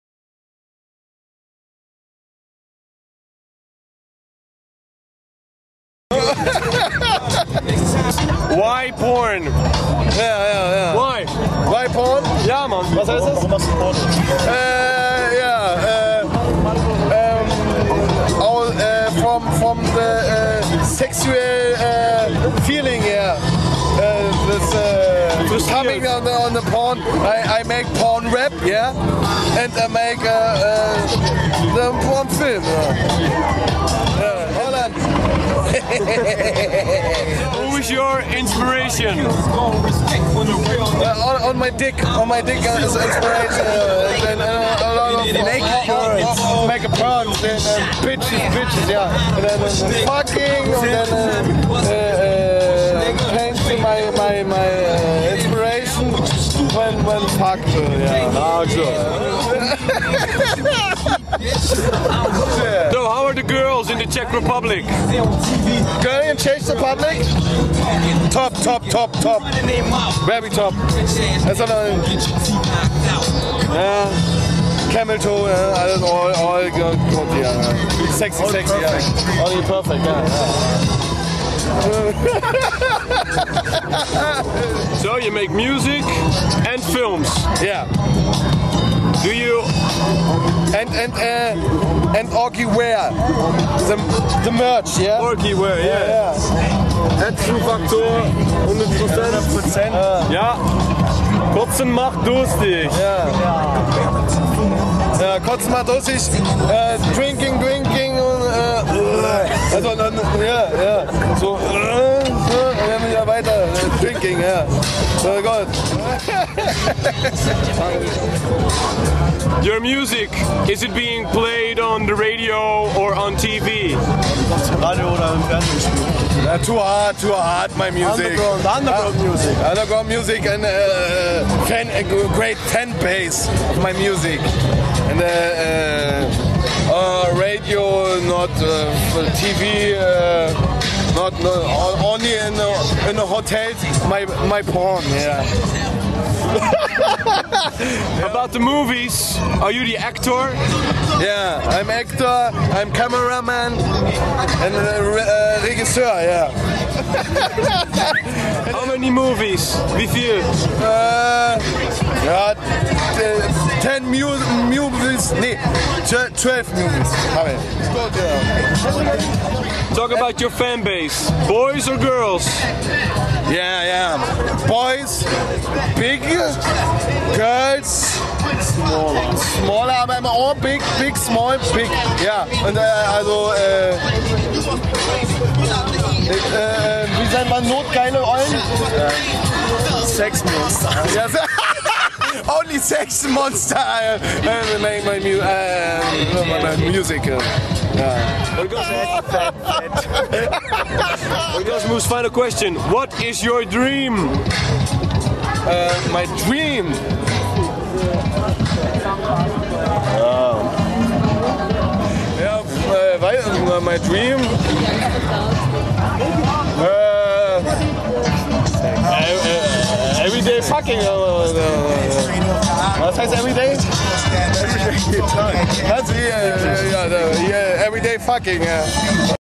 King Orgasmus: 'Mijn grootste droom is om iedere dag te neuken.'
Een van de meest kleurrijke MC's uit Duitsland is ongetwijfeld King Orgasmus. De porno rapper maakt muziek, films en heeft een eigen kledinglijn. Genoeg redenen om een erg serieus gesprek met deze man aan te gaan, nou ja serieus.